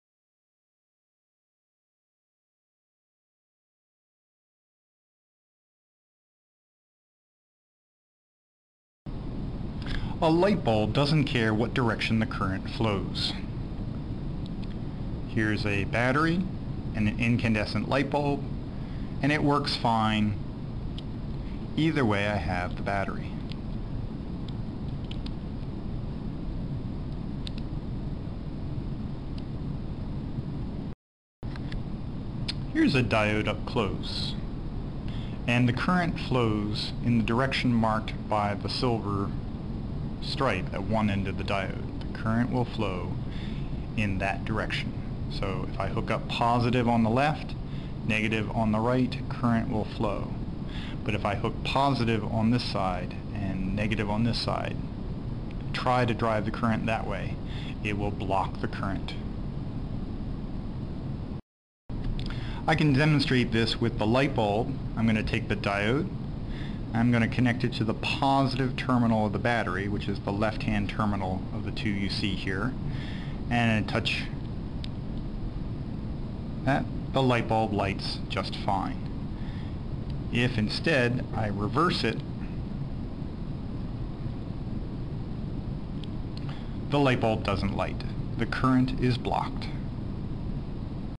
What are Diodes?
This video demonstrates explains what a diode is, and demonstrates one using a battery, lightbulb and (of course) a diode.